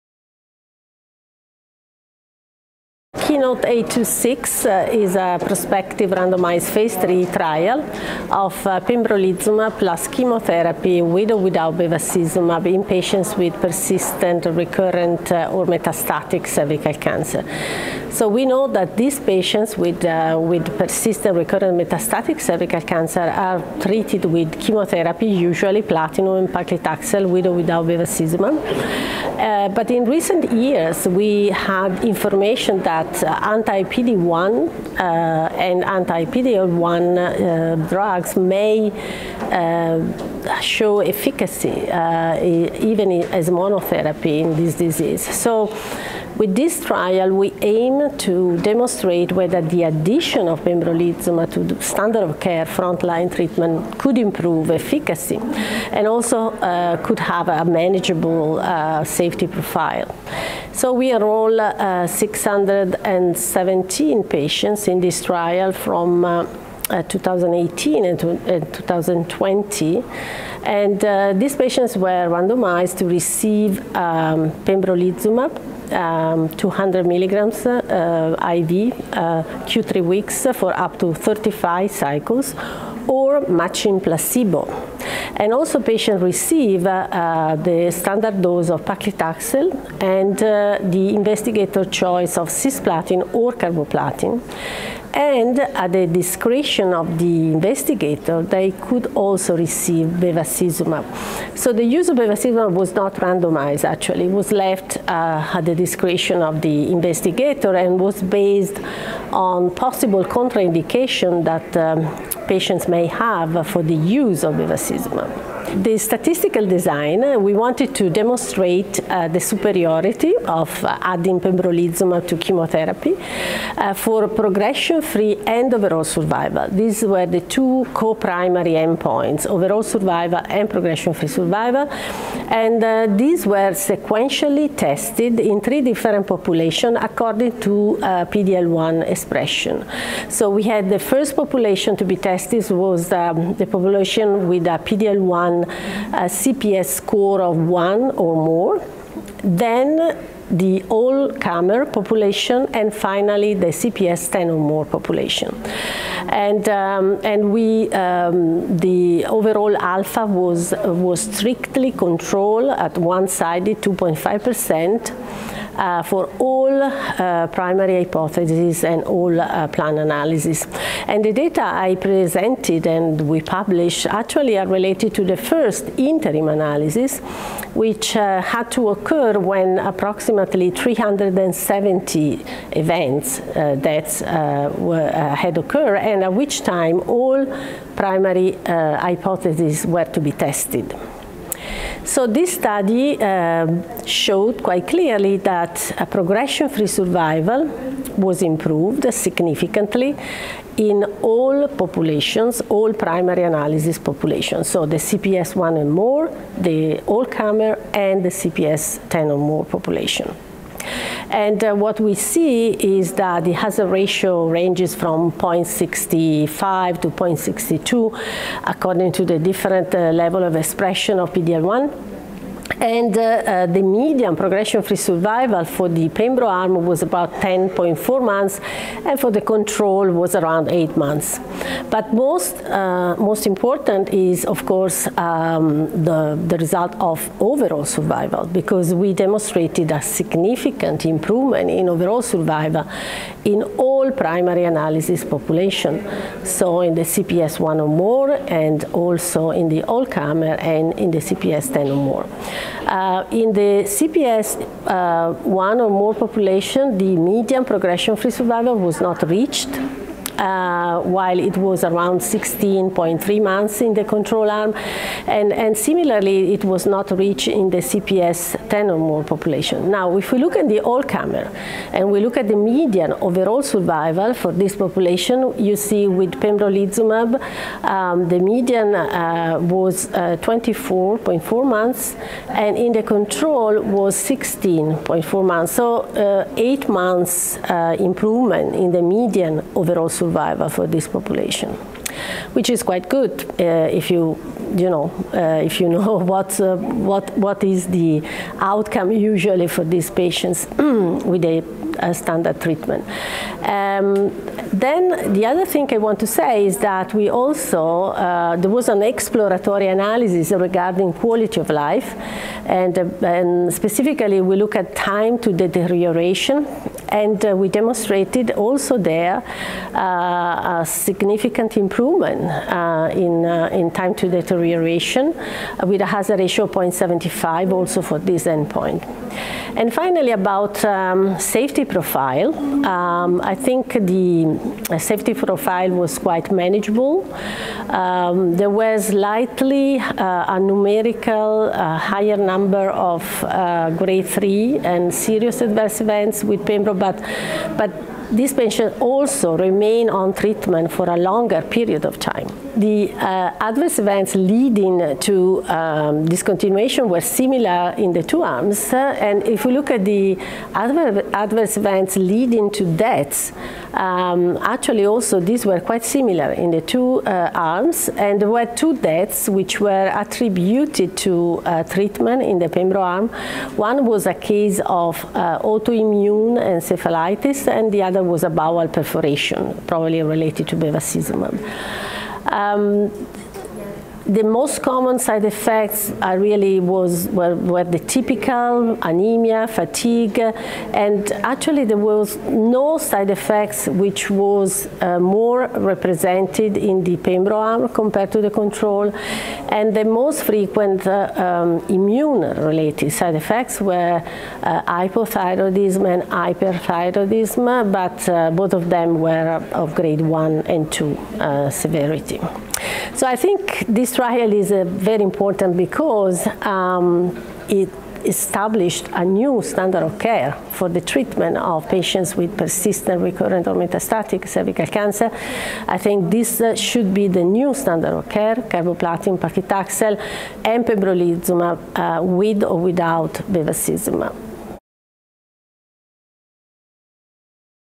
KEYNOTE-826: pembrolizumab + chemotherapy vs placebo + chemotherapy for cervical cancer
Nicoletta Colombo, MD, PhD, European Institute of Oncology, Milan, Italy & University of Milan-Bicocca, Milan, Italy, presents an update from KEYNOTE-826 (NCT03635567), a double-blind Phase III trial evaluating pembrolizumab versus placebo in patients with persistent, recurrent, or metastatic cervical cancer. As recently published, pembrolizumab in this patient population resulted in a significantly longer progression-free and overall survival. This interview took place at the European Society of Gynaecological Oncology (ESGO) 2021 congress in Prague.